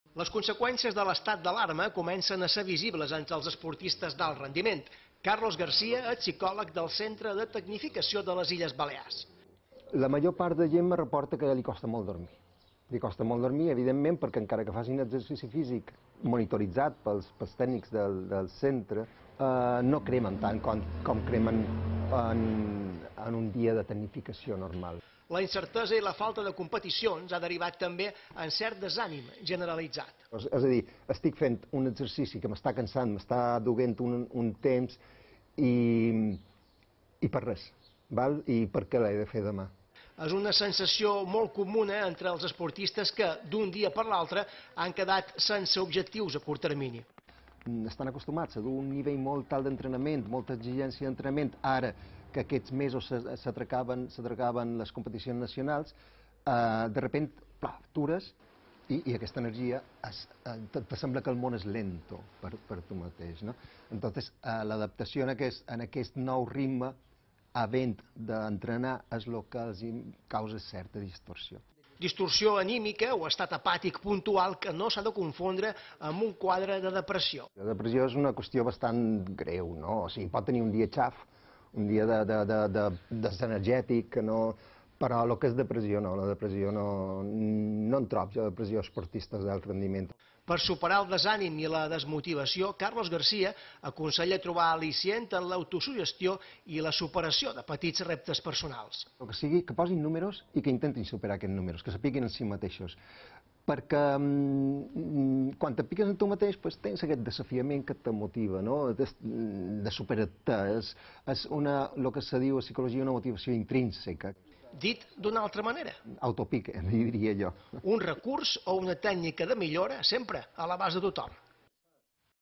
Creix la desmotivació entre els esportistes per l’estat d’alarma
La cancel·lació a totes les competicions esportives i la situació d’incertesa provocada per l’estat d’alarma pot derivar en desmotivació dels esportistes d’alt rendiment.

Sobre aquest tema hem parlat amb Carlos García, psicòleg del Centre de Tecnificació de les Illes Balears. García assegura que uns dels problemes més freqüents que es troba amb els esportistes és la falta de son pels vespres.

A més creu que la situació de confinament pot fer que els esportistes es desanimin i perdin les ganes de marcar-se nous objectius. Això sí, Carlos García avisa que aquesta situació no s’ha de confondre amb la depressió.